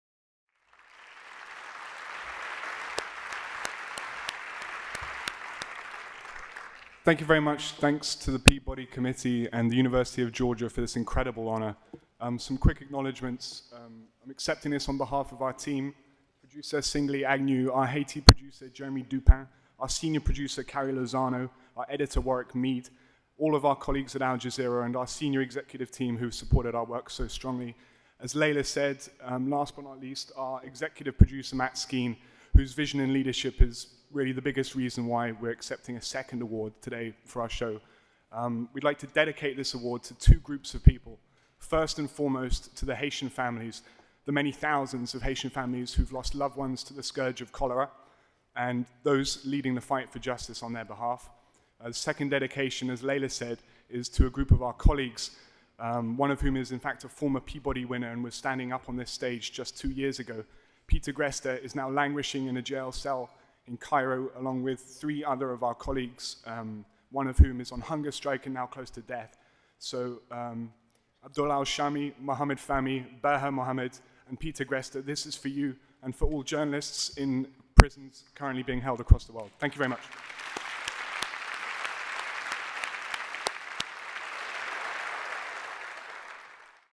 Sebastian Walker - Fault Lines: Haiti In a Time of Cholera - 2013 Peabody Award Acceptance Speech
Winner 2013 | Al Jazeera America

Following Haiti’s 2010 earthquake, a widespread cholera epidemic devastated an already shattered island nation, claiming nearly 8,000 lives and infecting more than half a million others. Al Jazeera America’s Fault Lines investigative team, led by reporter Sebastian Walker, cameraman Singeli Agnew and editor Warwick Meade, traveled to Haiti to follow the unfolding tragedy of this fatal illness. Their pursuit of accountability starts on the island but leads them to the United Nations’ headquarters in New York. Through relentless inquiry and a growing body of scientific evidence, they determined that the UN Stabilizing Mission peacekeepers almost certainly brought the disease into the country. Fault Lineschronicles the death and ongoing misery of cholera victims through its search for answers, while the people allegedly to blame for this outbreak are unwilling to acknowledge their responsibility. In the process, this investigative report provides victims with leverage in their fight for justice and opens avenues of redress for them. For its courageous investigation into an international health scandal, a Peabody Award goes to Fault Lines: Haiti in a time of Cholera.